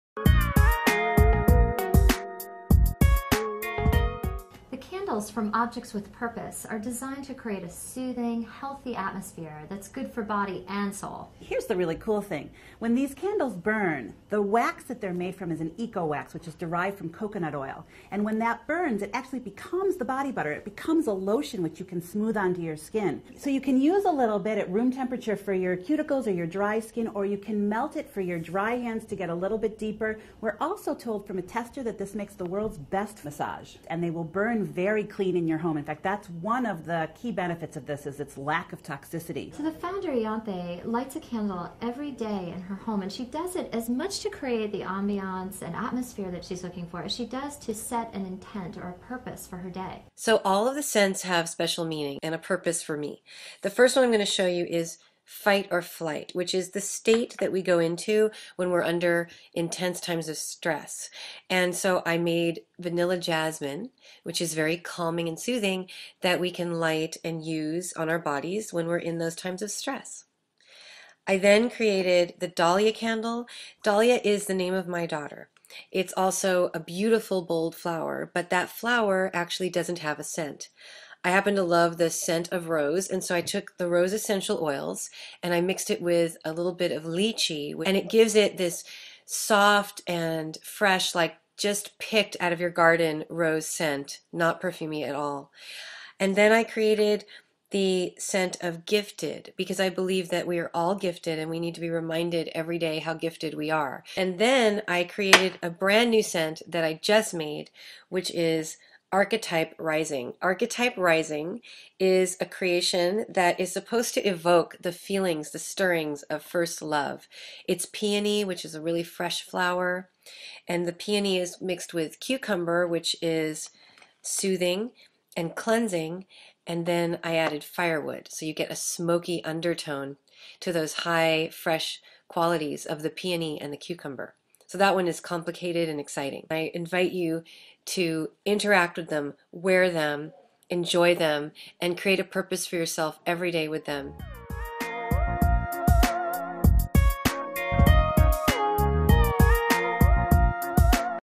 Objects with Purpose - Natural, Wearable Candle
The Grommet team discovers Objects with Purpose whose line of non toxic, organic candles create a wonderfully scented wax that can be used as a body moisturizer or perfume.